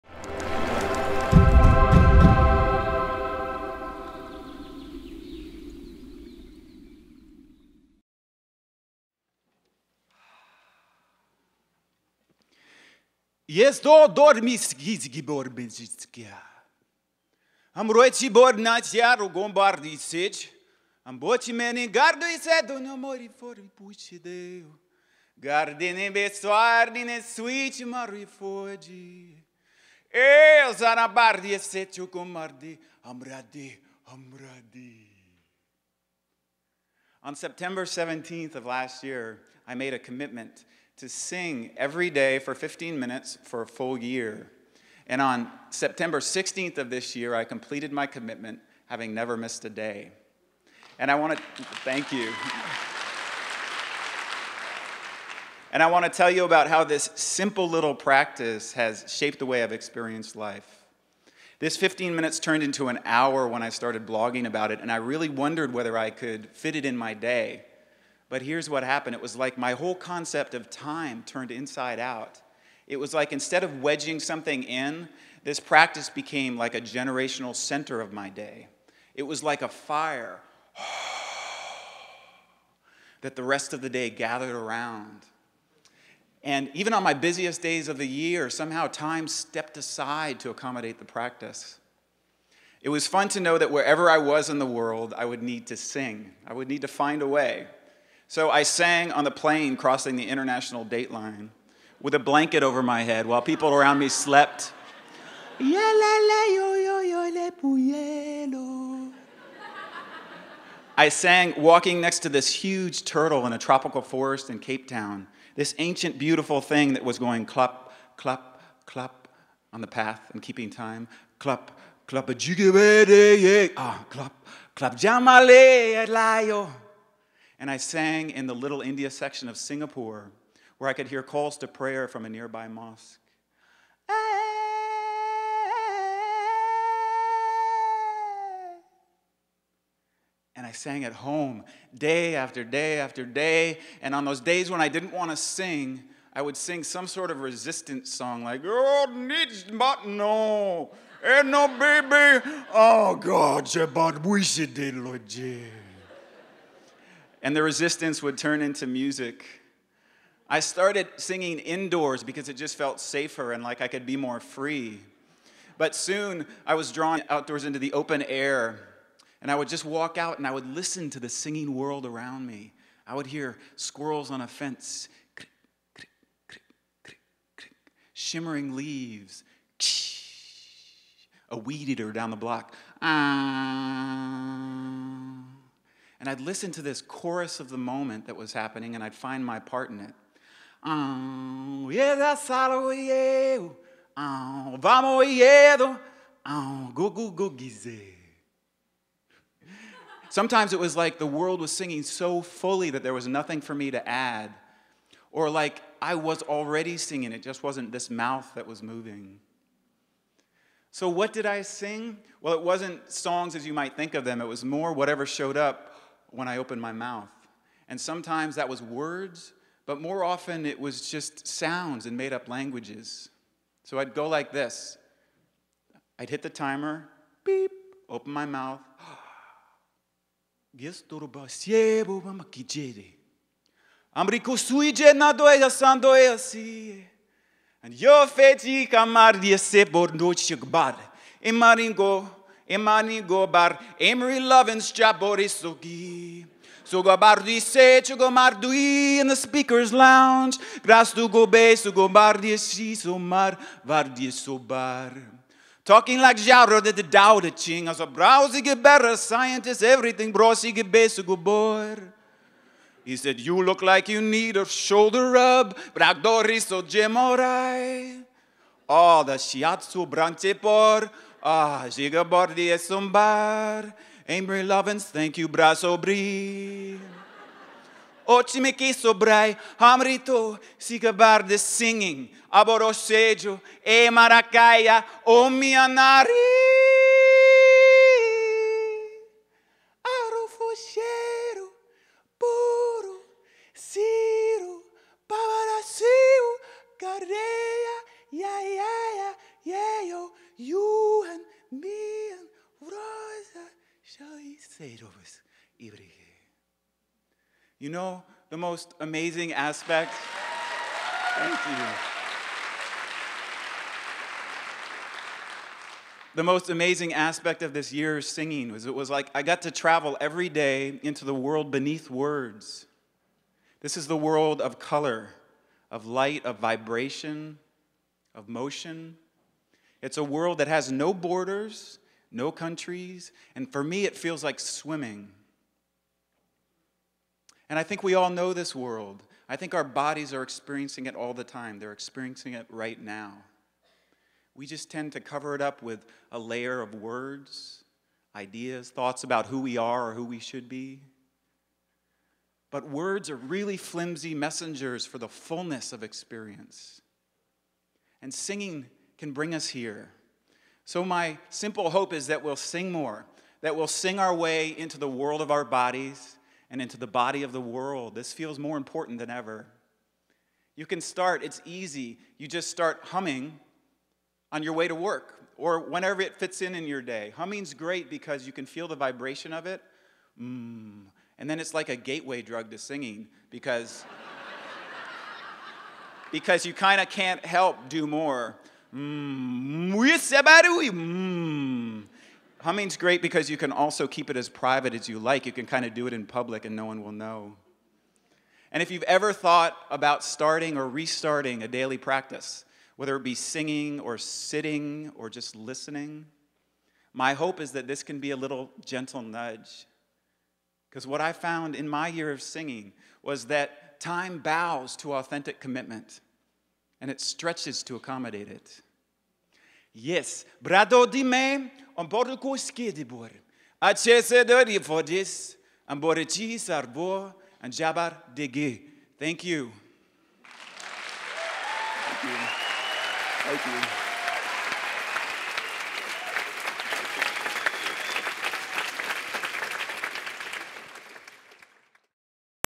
TEDxRainier - Will Hewett - Singing Yourself Alive
Vocalist and improviser Will Hewett explores the language of sound beneath words. After singing 15 minutes every day for a year, Will Hewett describes--and sings--what he learned about commitment and the ability of a simple creative practice to open us to vibrancy, and to a deeper connection with ourselves and the world around us.